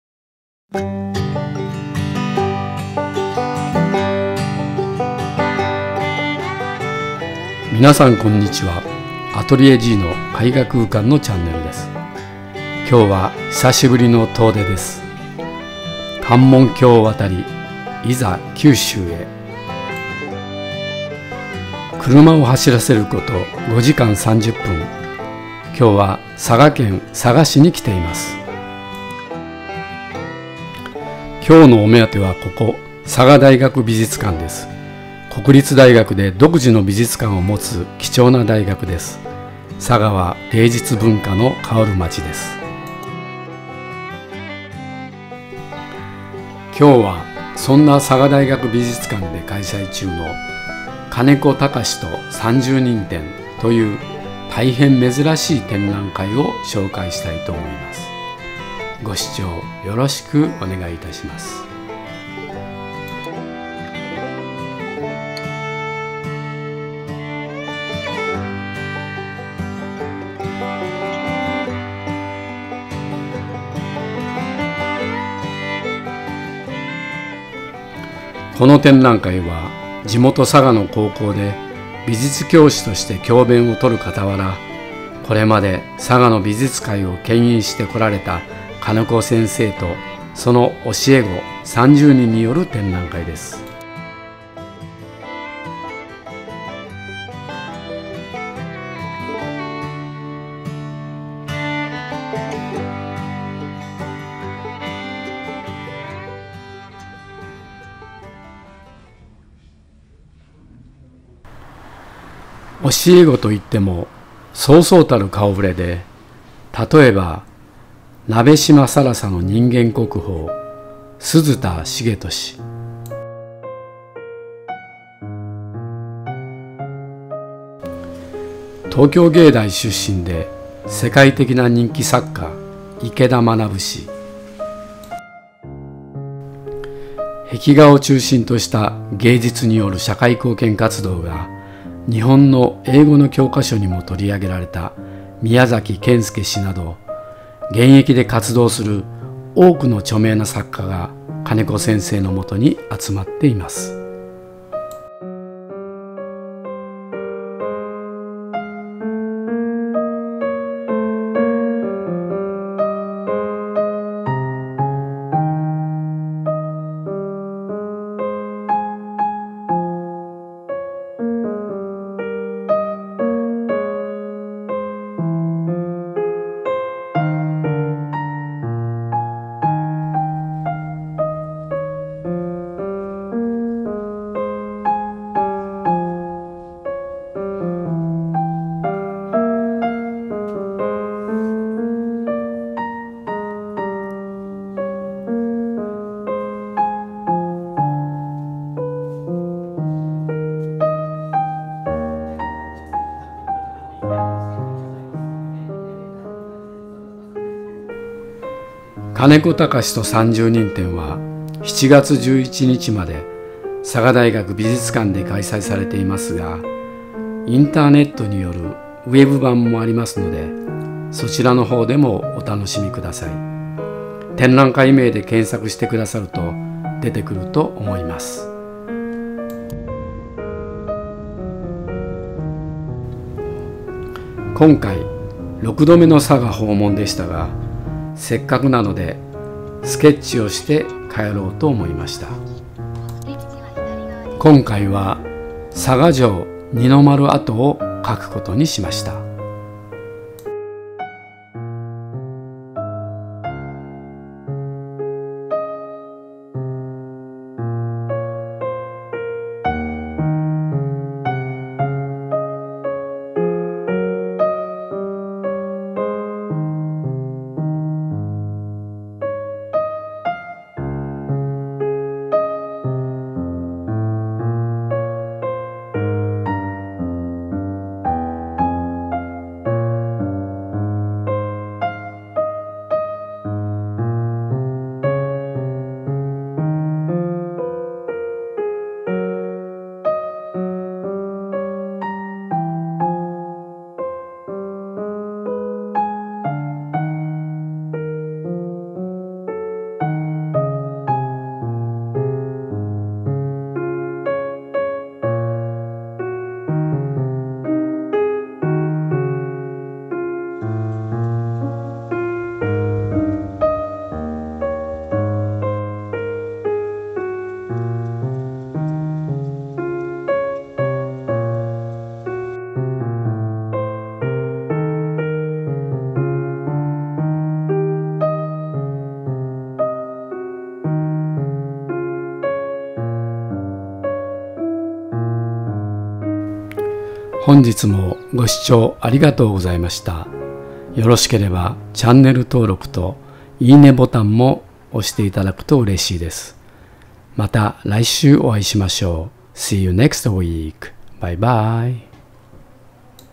【アトリエGの絵画空間】71展覧会紹介「佐賀大学美術館〜金子剛と三十人展」と佐賀のスケッチ
国立大学で独自に美術館を保有する珍しい大学、佐賀大学。私の知る限りでは東京芸大と佐賀大学にしか美術館は無いと思っています。なおかつ芸術大学ではなく、医学部などもある総合大学となると、唯一佐賀大学だけが美術館を有する大学となります。佐賀には昔から芸術文化を大切にする風土があったのではないかと思います。1864年のパリ万博はジャポニズムのきっかけとなる博覧会でしたが、佐賀藩は薩摩藩とともに幕府を凌ぐ数の物産品を西洋諸国に披露しています。現在のアニメブームのような日本文化の発信が佐賀の人々によって行われていたことに今一度注目すべきだと考えています。そんな佐賀の地で開催の「金子剛と三十人展」はまさに佐賀の文化を象徴したような展覧会だと感じました。おヒマな時にご視聴賜れば幸いです。